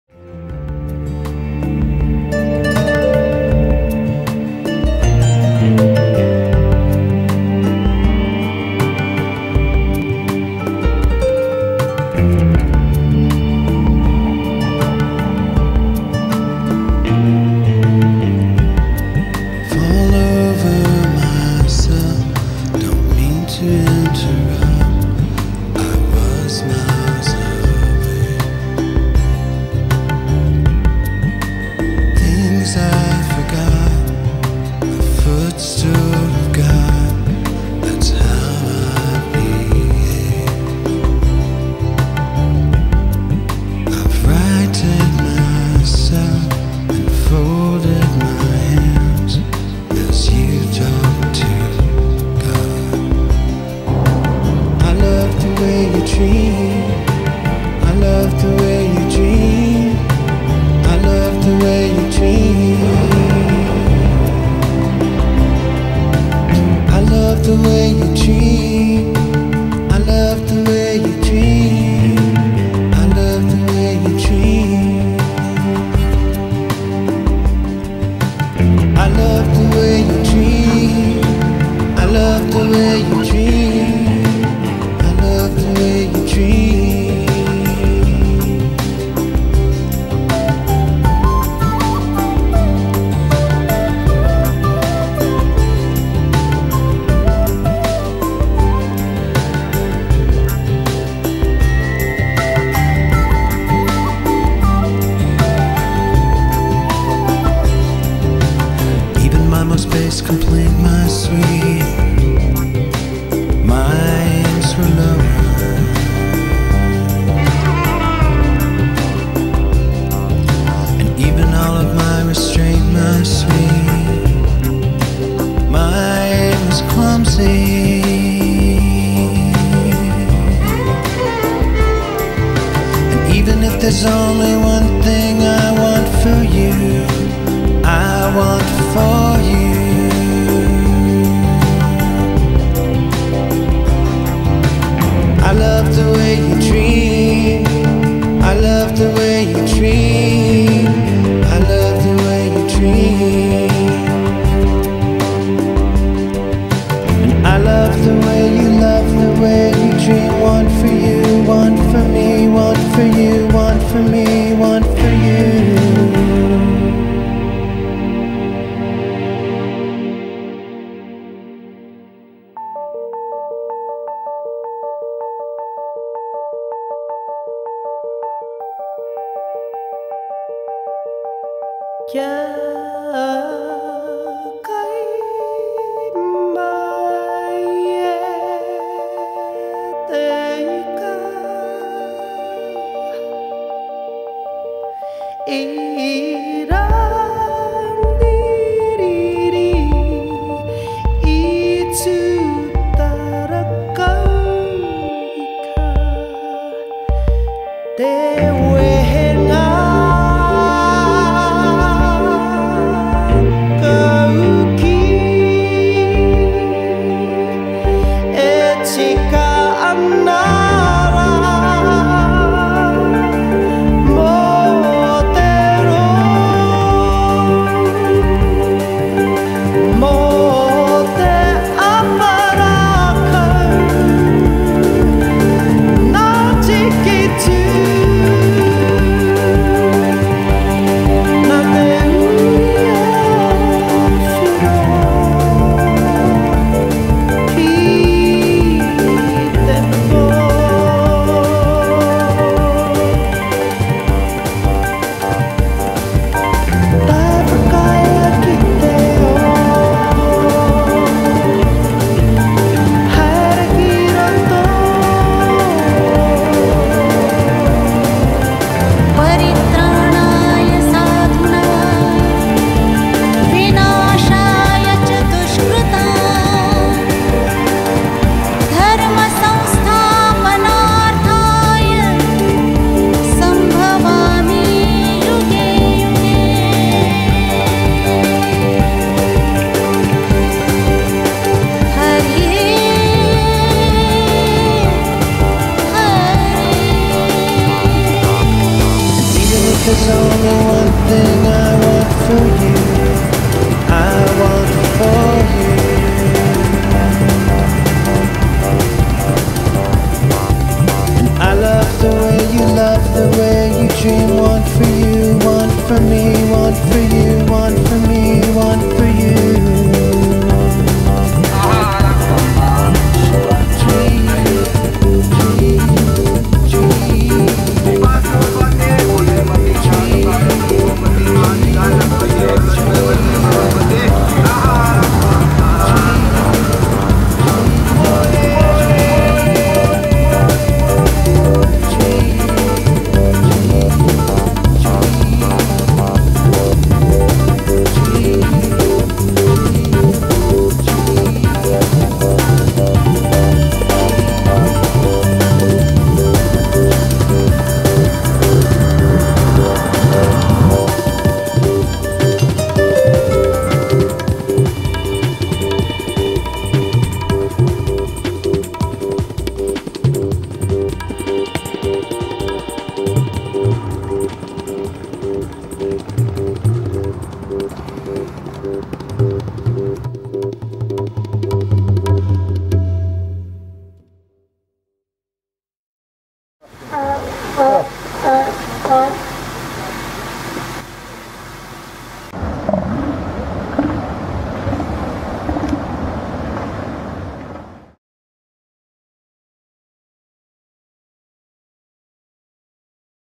Morro Bay
Inspired by the music of Michael Stipe, my latest video "Morro Bay" gives the viewer a first-hand look at a few beautiful Pacific Ocean towns, as well as Sea Lions, Sea Otters, Seals, along with a myriad of ocean wildlife and coastal sunsets.. Filmed in December 2014, including location shots from Cambria and Cayucos, California.